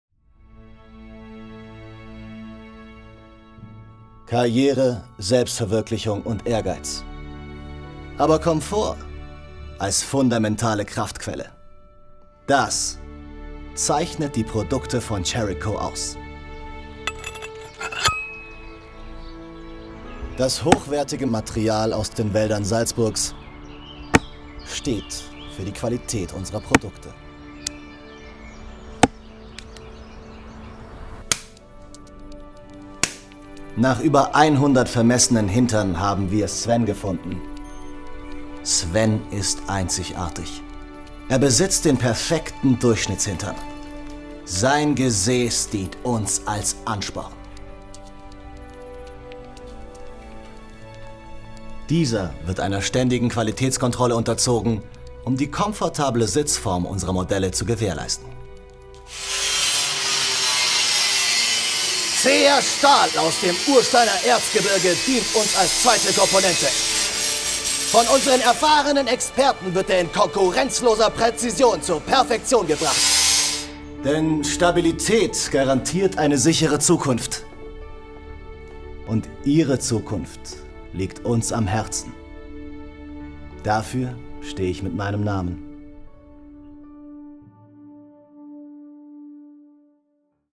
Chairicho Spec Commercial
Der fiktive Imagespot "Chairico" bewirbt ein Firma, welche laut Eigendarstellung führend in der Produktion von Sitzgelegenheiten ist. Der Spot entstand im Rahmen eines Licht & Kamera Workshops unter der Leitung von Stephan Vorbrugg. Innerhalb von zwei Tagen wurde der Spot in Full HD mit einer Arri Alexa Cinema Kamera umgesetzt. Ziel war es einerseits die Licht-Situationen professioneller Produktionen zu emulieren sie dabei aber gleichzeitig zu persiflieren.
 Dominik Bücheler
MMA Bachelor 2012
Aufgaben: Kamera
Markus Wastl
Markus Wastl
MMA Bachelor 2012
Aufgaben: Kamera
Oliver Valentin Bernauer
Oliver Valentin Bernauer
MMA Bachelor 2012
Aufgaben: Regie
Martin Benedikt Hormann
Martin Benedikt Hormann
MMA Bachelor 2012
Aufgaben: Regie
Amadeus Karl Zajac
Amadeus Karl Zajac
MMA Bachelor 2012
Aufgaben: Produktion
Bianca Bujic
Bianca Bujic
MMA Bachelor 2012
Aufgaben: Produktion, Drehbuch, Continuity
Markus Kendlbacher
Markus Kendlbacher
MMA Bachelor 2012
Aufgaben: Produktion
Alexander Christian Wallner
Alexander Christian Wallner
MMA Bachelor 2012
Aufgaben:
Jakob Schweighardt
Jakob Schweighardt
MMA Bachelor 2011
Aufgaben:
Andreas Helmut Hornauer
Andreas Helmut Hornauer
MMA Bachelor 2012
Aufgaben:
Jonas Wenzl
Jonas Wenzl
MMA Bachelor 2012
Aufgaben: Schnitt
André Leubner
André Leubner
MMA Bachelor 2012
Aufgaben: Aufnahmeleitung
Steffen Schlegel
Steffen Schlegel
MMA Bachelor 2012
Aufgaben: Aufnahmeleitung
Pascal Hübner
Pascal Hübner
MMA Bachelor 2012
Aufgaben: Lichtsetzung
Gregor Müller
Gregor Müller
MMA Bachelor 2012
Aufgaben: Lichtsetzung
Anna Elisabeth Wiechers
Anna Elisabeth Wiechers
MMA Bachelor 2012
Aufgaben: Ausstattung
Sandra Lanzl
Sandra Lanzl
MMA Bachelor 2012
Aufgaben: Set Runner
Felix Maringer
Felix Maringer
MMA Bachelor 2012
Aufgaben: Komposition, Tonschnitt, Audio Mastering
Tino Schleinitz
Tino Schleinitz
MMA Bachelor 2012